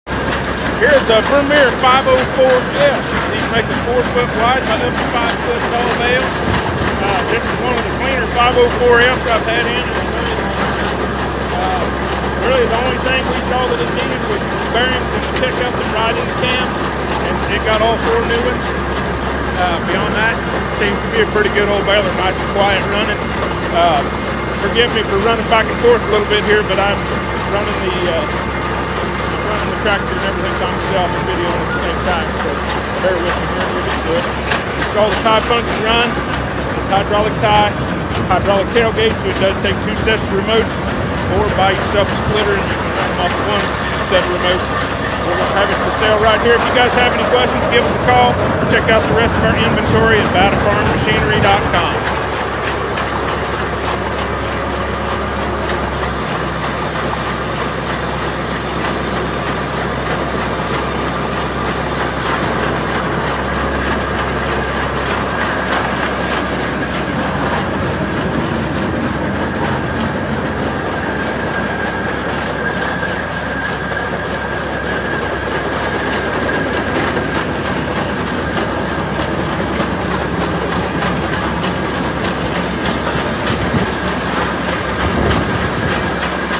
VERMEER 504F For Sale
4x5 bale, hydraulic tie and tailgate, needs 2 sets of remotes, new bearings. Watch the video for more information.  Fresh Round Balers